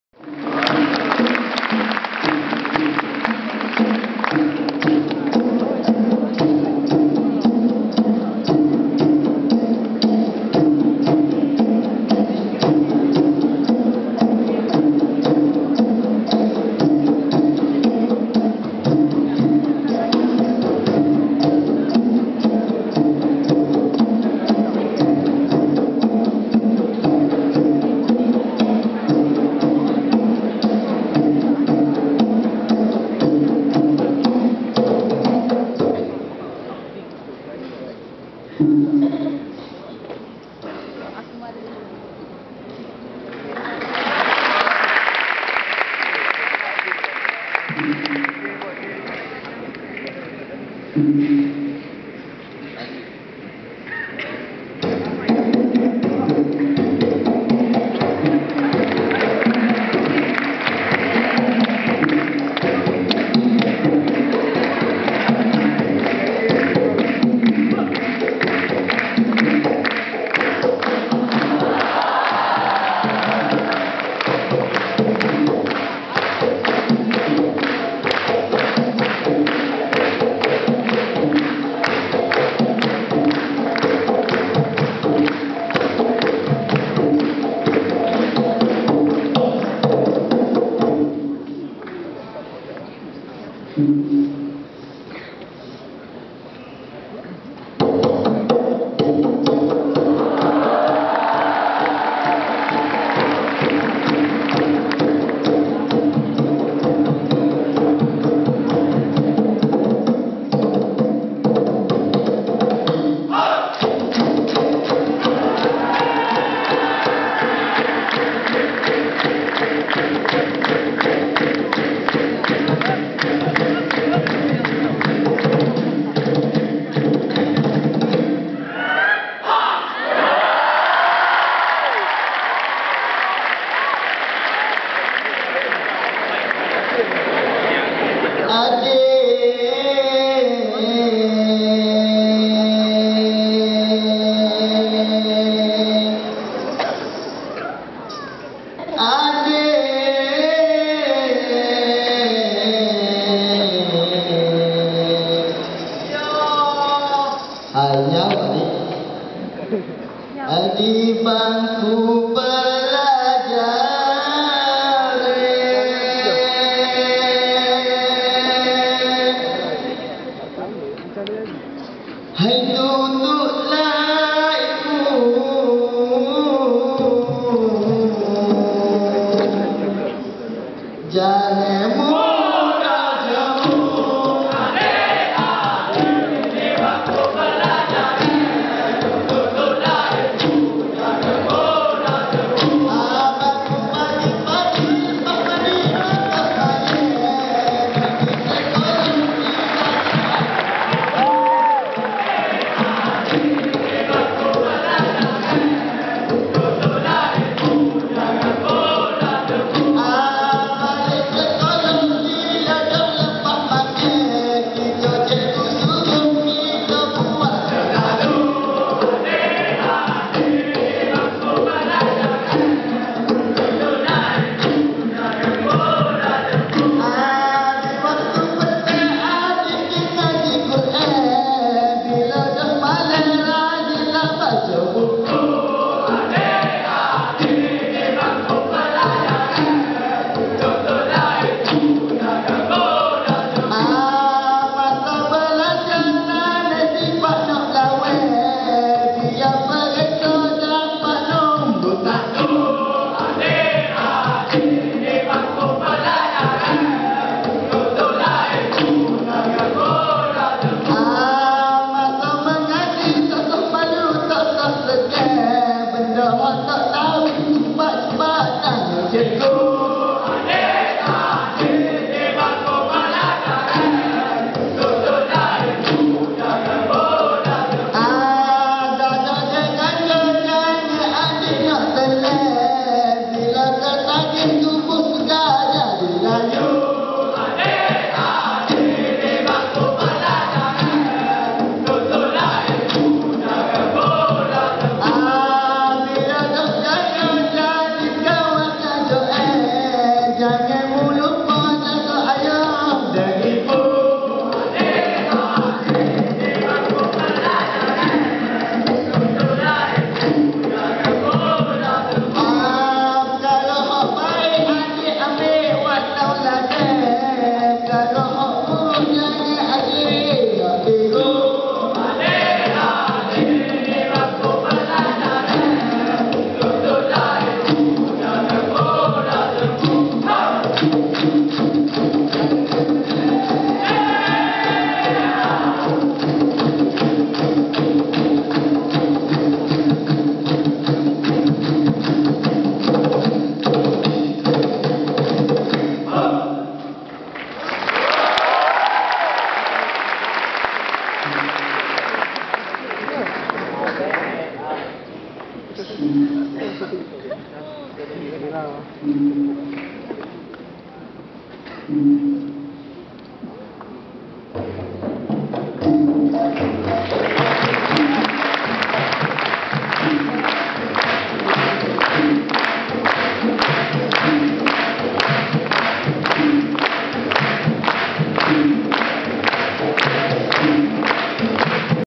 Dikir Barat Malam Persembahan MHS 2012/13
Dikir Barat performance by pelajar2 PASUM 12/13